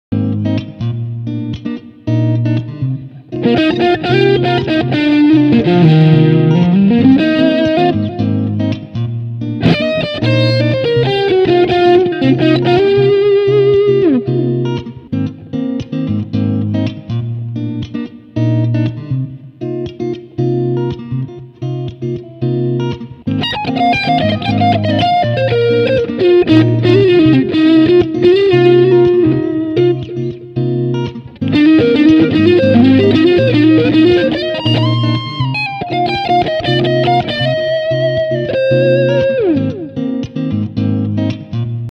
Tone test 2. DiMarzio paf master
Testing with one of my overdrive patches I saved on my POD HD500. Created a short looped progression in Eb Major.